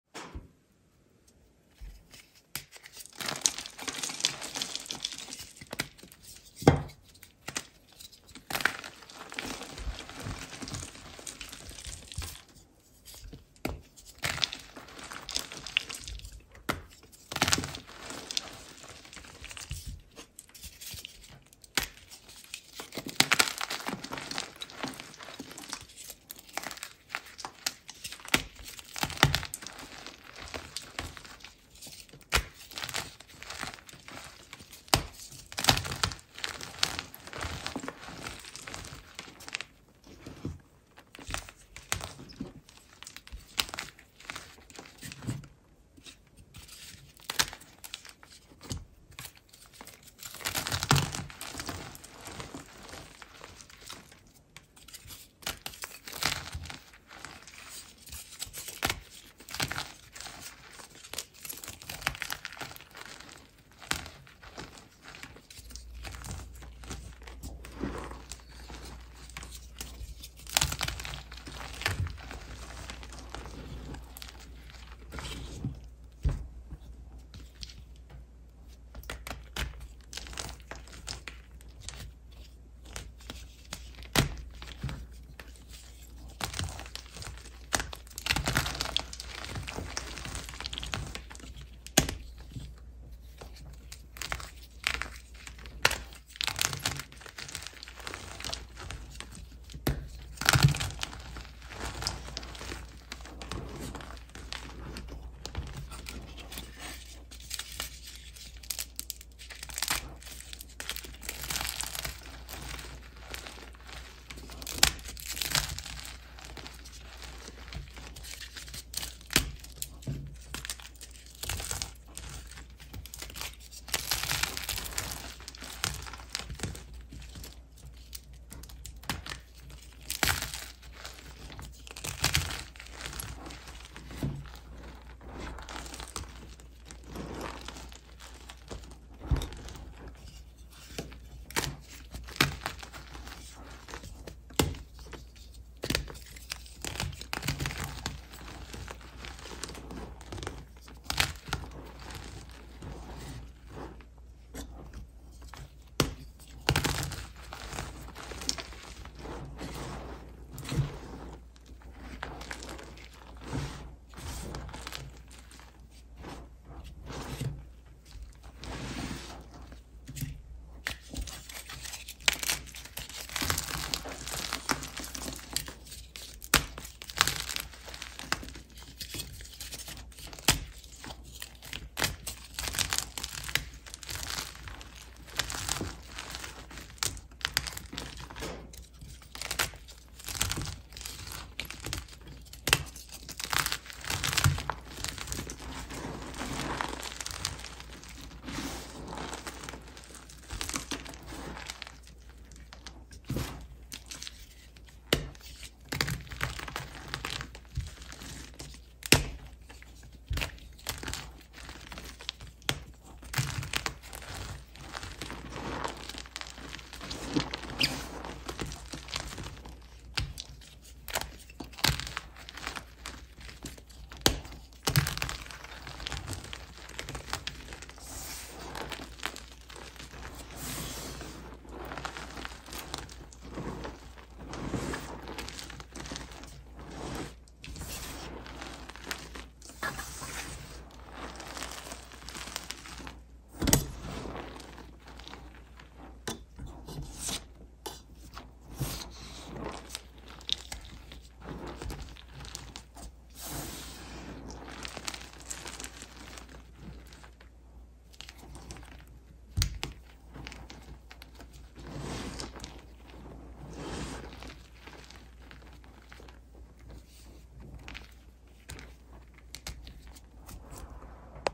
Deep Dyed Gym Chalk Crush
I used a whole bottle of dye on each block of chalk 🥹

Such an expensive crush but worth it!! 🥰❤️